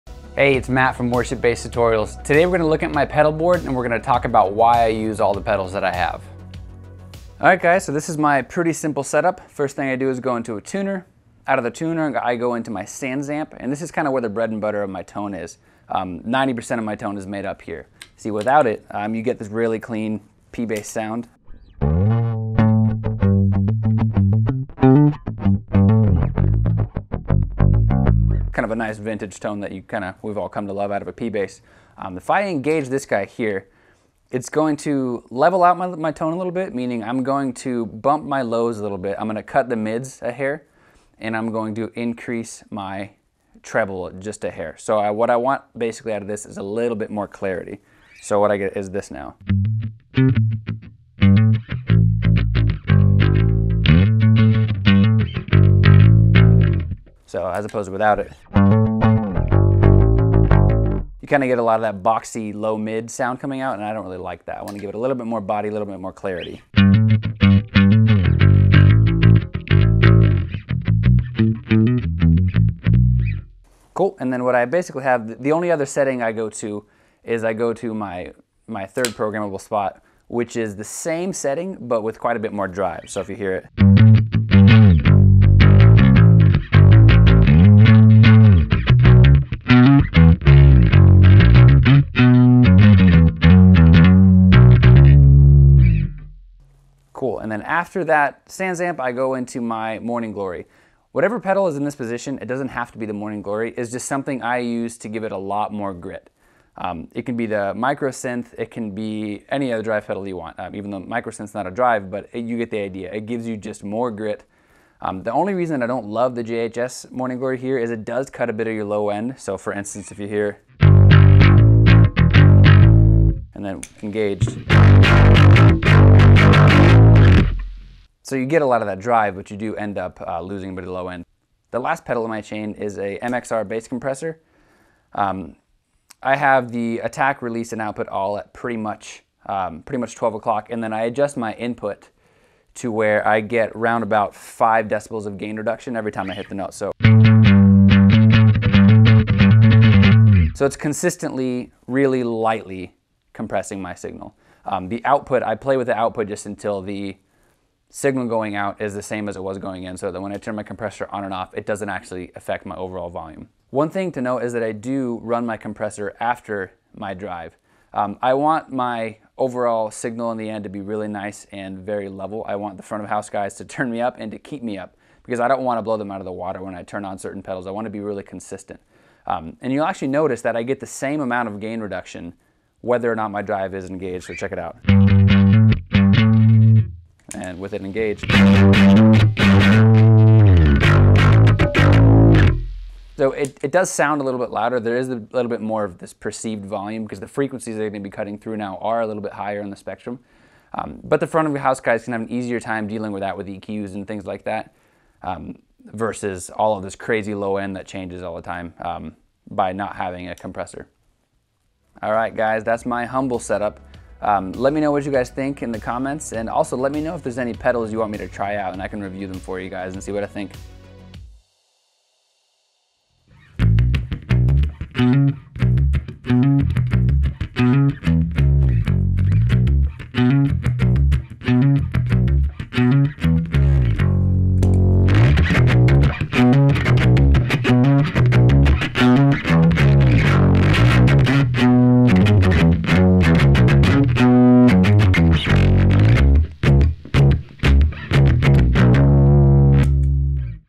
Worship Bass Pedalboard/Tone Overview
A quick overview of all of the pedals I use and why they have earned a place on my pedalboard.

Pedals:
Tech21 SansAmp Programmable Bass DI
JHS Morning Glory
MXR Bass Compressor
TC Electronic Polytune 2 Mini

Pedalboard:
Temple Audio SOLO 18

Mics:
AKG P220
Shure SM57

Other:
Universal Audio Apollo Twin
Focusrite Scarlett 18i20

Camera Gear:
Canon T7i/800D
Canon 50mm Lense